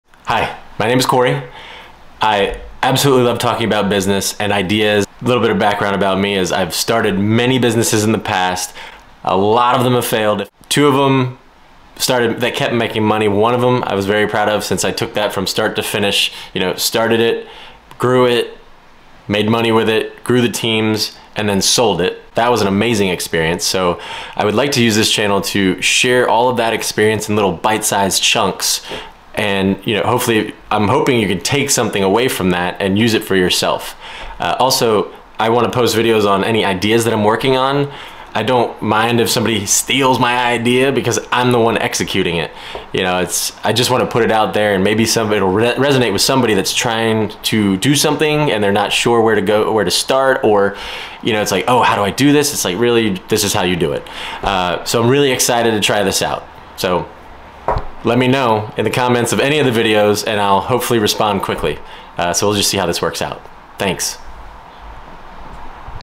Channel Introduction
This is a quick intro video to understand my experience and why im doing this channel for now (this will be improved upon, but just had to get started!)